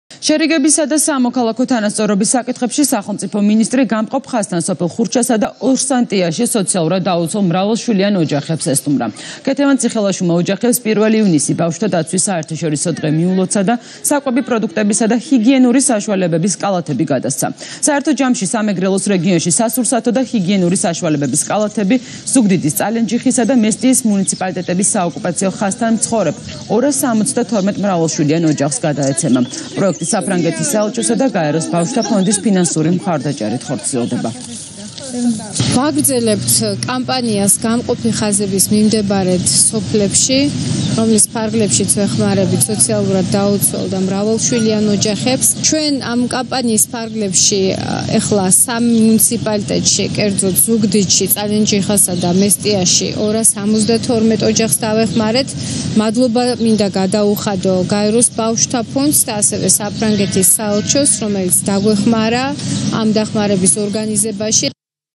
ბავშვთა დაცვის დღე საოკუპაციო ხაზთან
შერიგებისა და სამოქალაქო თანასწორობის საკითხებში სახელმწიფო მინისტრი ქეთევან ციხელაშვილი, სამეგრელო-ზემო სვანეთის სახელმწიფო რწმუნებულის მოადგილესთან ნინო ვიბლიანთან და ზუგდიდის მერთან გიორგი შენგელიასთან ერთად, გამყოფ ხაზთან სოფელ ხურჩასა და ორსანტიაში სოციალურად დაუცველ მრავალშვილიან ოჯახებს ესტუმრა. მინისტრმა ოჯახებს 1 ივნისი, ბავშვთა დაცვის საერთაშორისო დღე მიულოცა და საკვები პროდუქტებისა და ჰიგიენური საშუალებების კალათები გადასცა.

საერთო ჯამში რეგიონში სასურსათო და ჰიგიენური საშუალებების კალათები ზუგდიდის, წალენჯიხისა და მესტიის მუნიციპალიტეტების საოკუპაციო ხაზთან მცხოვრებ 272 მრავალშვილიან ოჯახს გადაეცემა. პროექტი საფრანგეთის საელჩოსა და გაეროს ბავშვთა ფონდის ფინანსური მხარდაჭერით ხორციელდება.